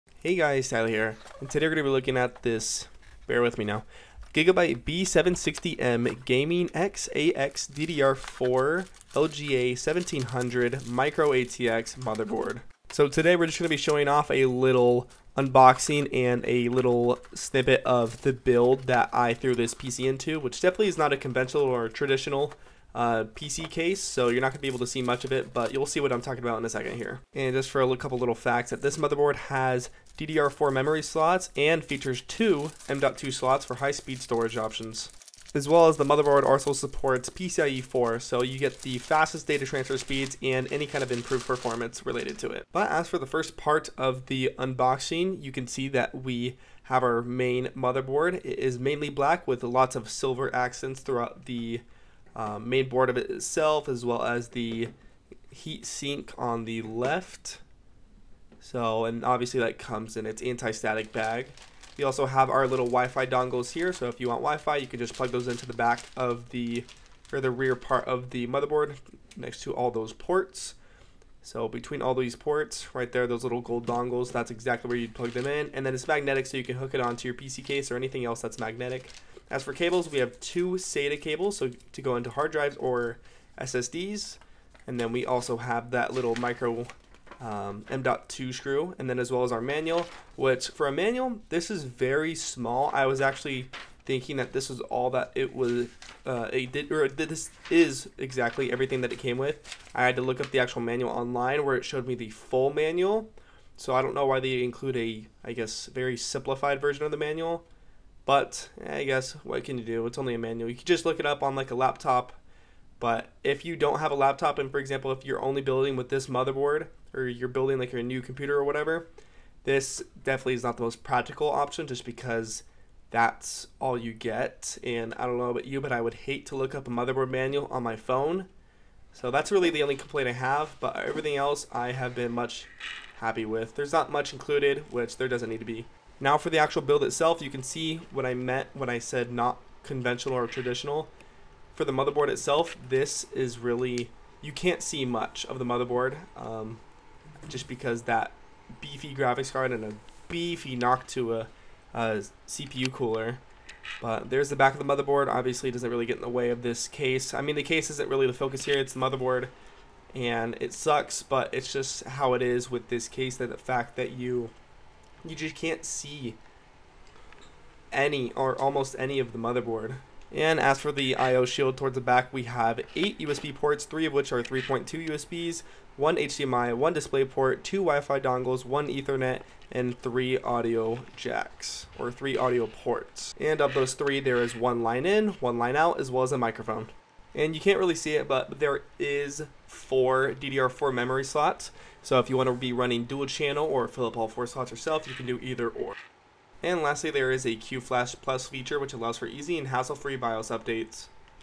Tech Marvel: GIGABYTE B760M Analysis & Unboxing
GIGABYTE B760M Gaming X AX DDR4 (LGA 1700/ Intel/ B760/ M-ATX/ DDR4/ 2* M.2/ PCIe 4.0/ Front USB 3.2 Gen 2 Type-C/ 2.5GbE LAN/Q-Flash Plus/PCIe EZ-Latch/Gaming Motherboard)